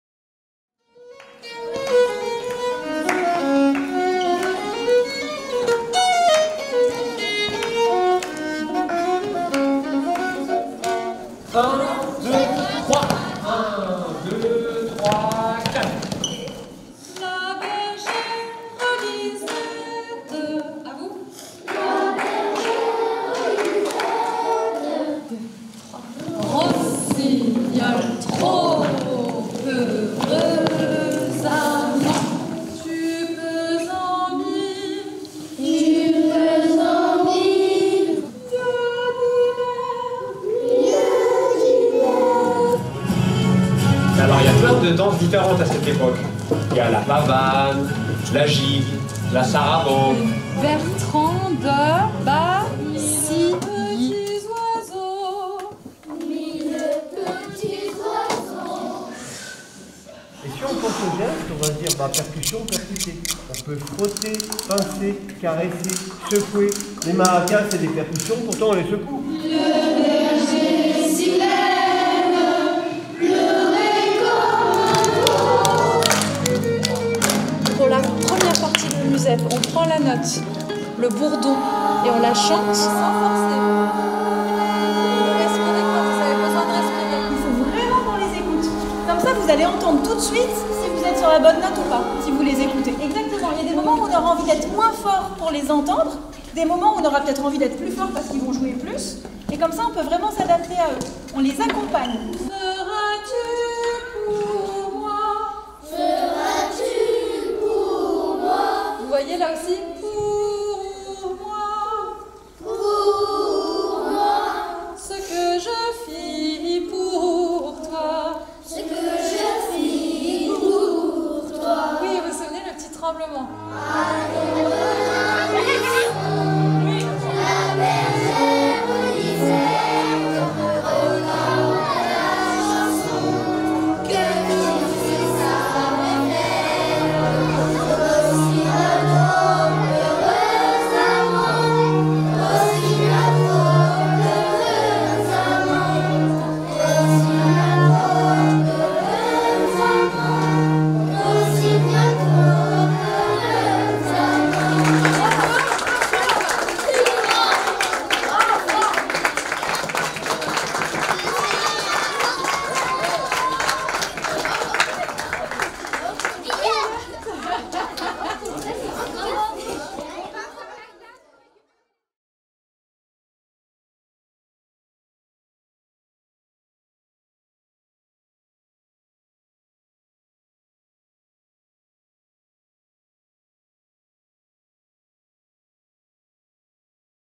Le Bois qui Danse - Éducation Artistique et Culturelle - La Verrière - Comet Musicke
Comet Musicke - Early Musicke Ensemble - A storytelling

En partenariat avec le Centre de Musique Baroque de Versailles (CMBV), Comet Musicke a accompagné pendant une année une classe de CE2/CM1 de l'école du Bois de l’Étang à La Verrière (78) avec comme objectif la restitution en fin d'année d'un petit spectacle public comprenant notamment trois chansons (airs de cour) de Bertrand Bacilly (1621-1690). Durant les ateliers préparatoires : apprentissage de notions élémentaires de musique, compréhension de la musique baroque, travail vocal et corporel, danse, dessin, écoute, visionnage, présentation des instruments, travail de la caméra, etc.

Un film de Sylvain Trousselle, avec :
Les élèves de l'école du Bois de l’Étang (La Verrière)
Aude-Marie Piloz            viole de gambe, chant
Marie Favier                    mezzo-soprano
Camille Rancière            violon, chant
Cyrille Métivier               violon, chant
Axel Lecourt                   instruments traditionnels du monde

Remerciements
Centre de Musique Baroque de Versailles (CMBV)
Flûte, c'est une Clef d'Ut